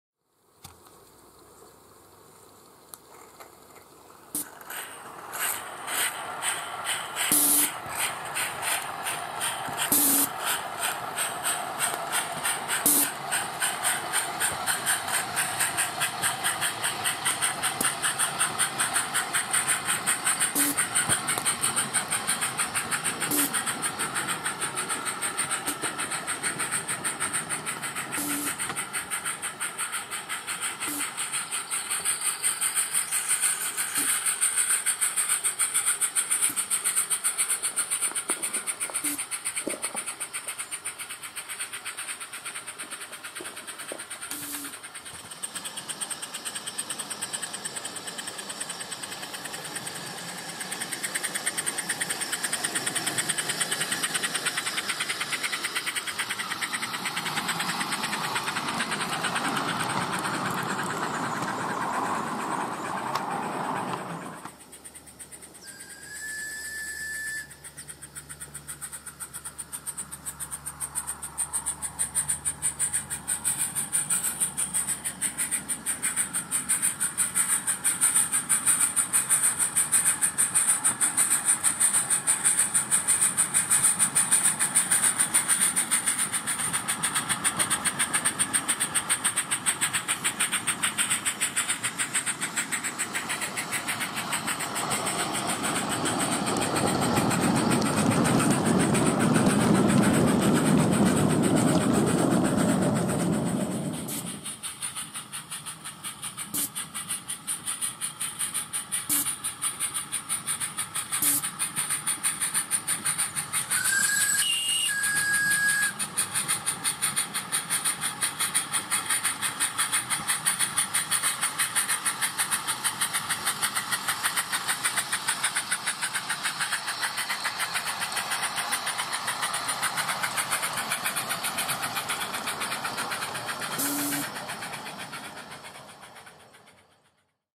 Sand Hutton No.10 July 15
More Slomo and Summerlands Chuffer. This time with a Slomo in a coal truck as well as the one in the loco. Gives even more control and chuff!